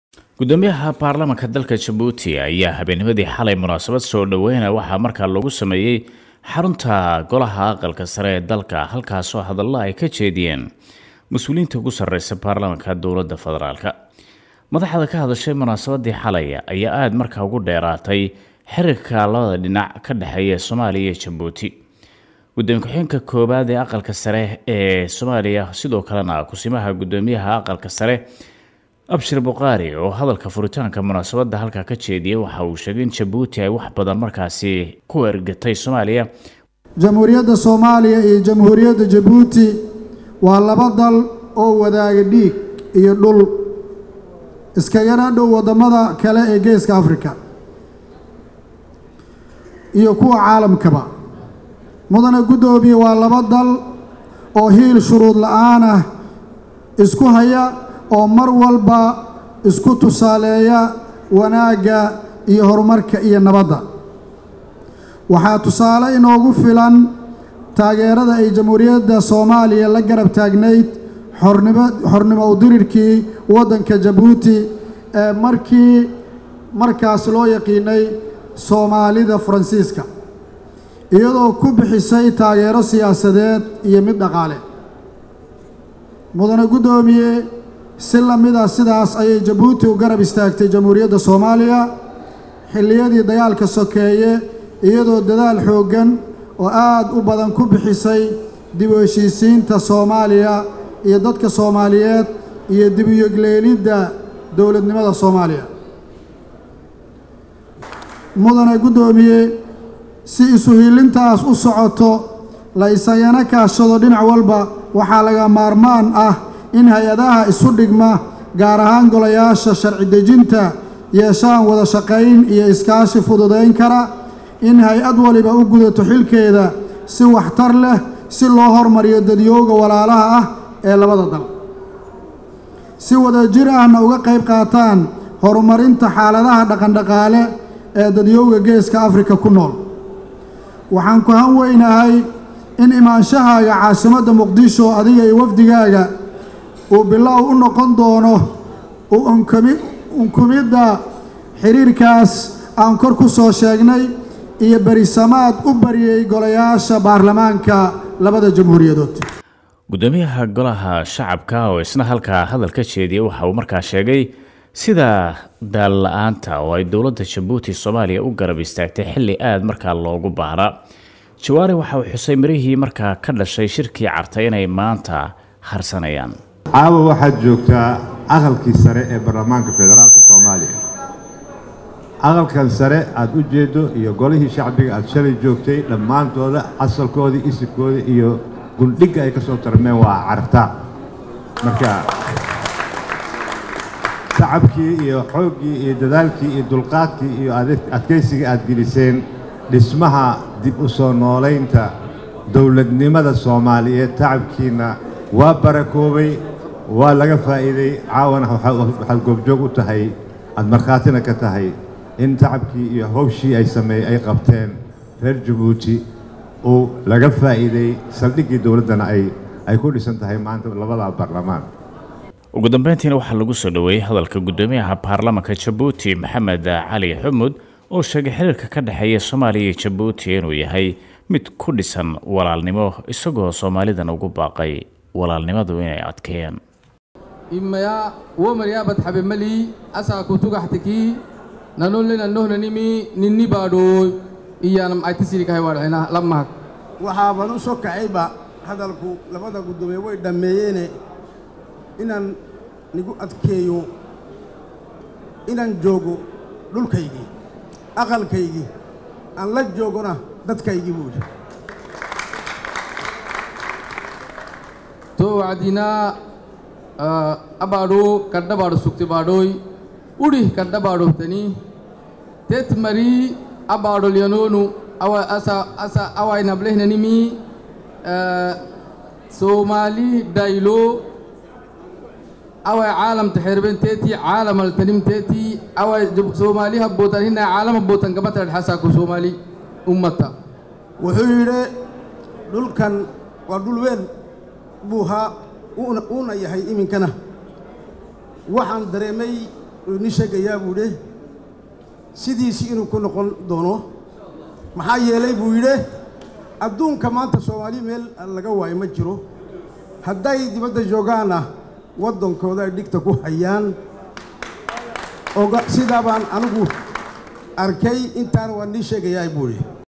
GOLAHA AQALKA SARE OO SOO DHAWEYN U SAMEEYAY WAFDIGII GUDOOMIYAHA
GOLAHA  AQALKA SARE OO SOO DHAWEYN U SAMEEYAY WAFDIGII GUDOOMIYAHA BARLAMAANKA  JABUUTI